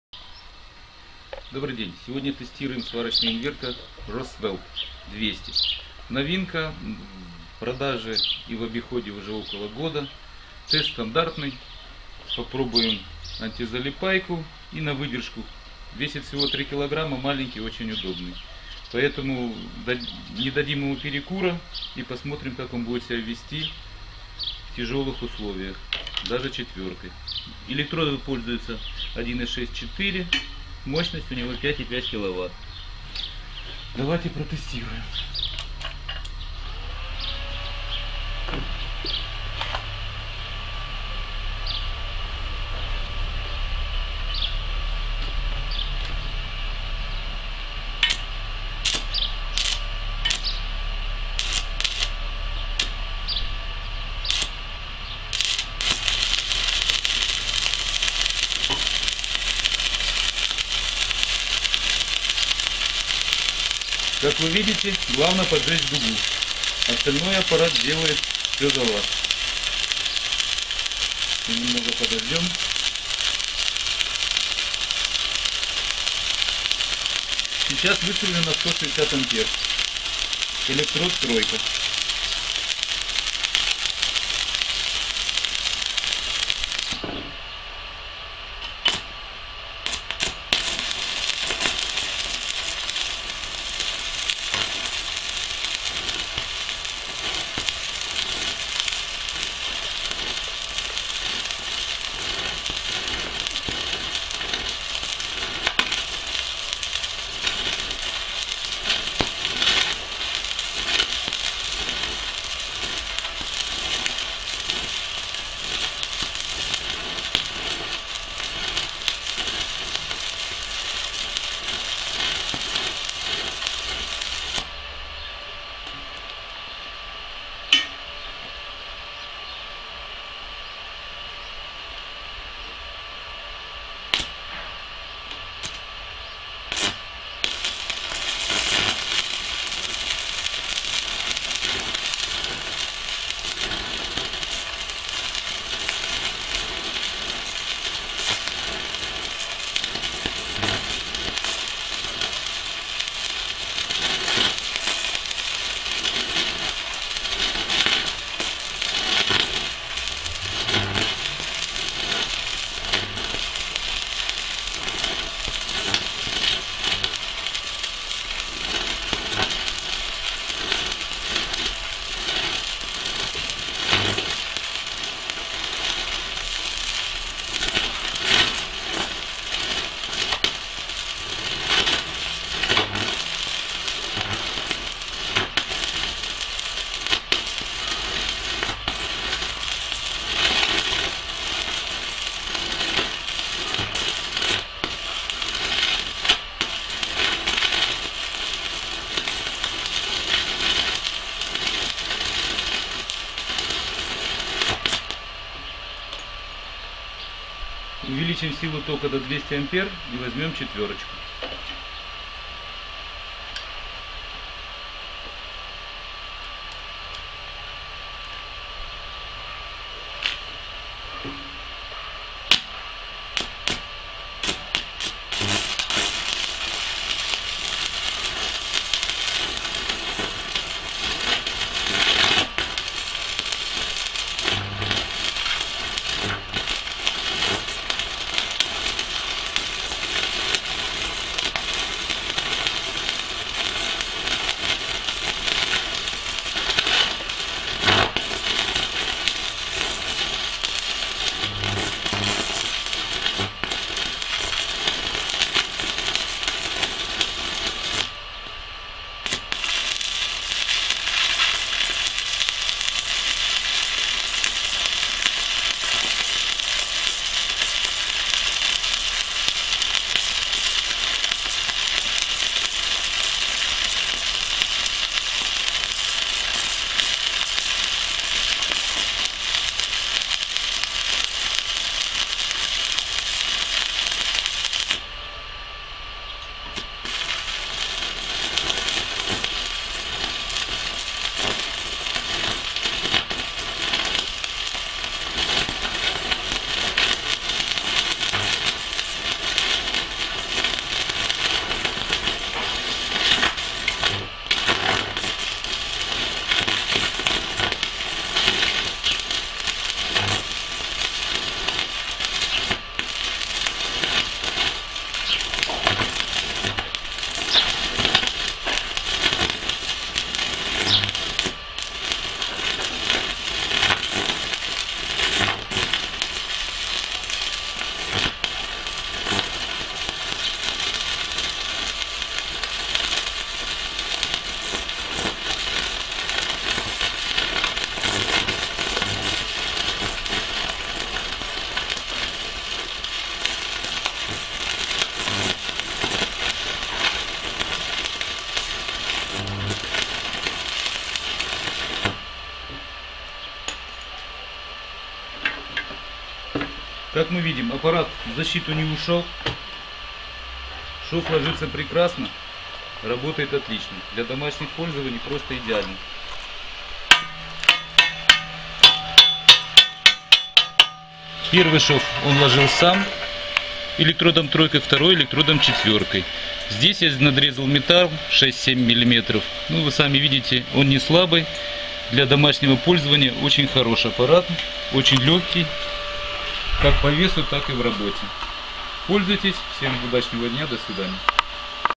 Самый маленький сварочный инвертор для дома Pocweld MMA 200 mini Поквелд
Если вам нужен аппарат не дорогой компактный, что для частного сектора будет достаточно, то выбор очевиден. Так как габариты этого инвертора достаточно малы и вес в 3 кг даст возможность как в хранение так и в работе.
Видео клип показывает очень хорошие результаты в работе и при нагрузке.
Сервисные центры по ремонту находятся по все большим городам Украины. Во время работы оператор использует для защиты маску сварщика хамелеон Optex 777 ( Оптех 777 ) благодаря чему работа с таким инвертором становиться очень простой даже для новичка.
Остальные данные о сварочном инверторе РОСweld находятся на сайте указанным при видео. Пользуйтесь и берегите здоровье, Всем удачного дня.